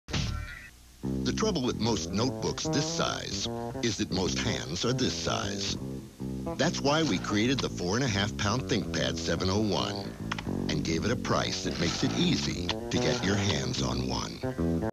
IBM ThinkPad 701 Commercial (1995)
Television commercial for the then-innovative IBM ThinkPad 701 laptop computer, which had a unique fold-out keyboard. (1995)